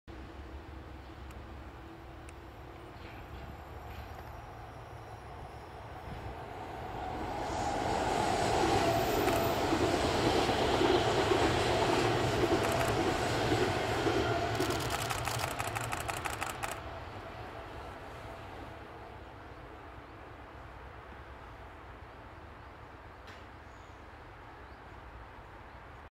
小田急・桜のトンネル×LSE（2）―Spring Odakyu romancecar (2)
小田急の桜のトンネルを駆け抜けるLSE。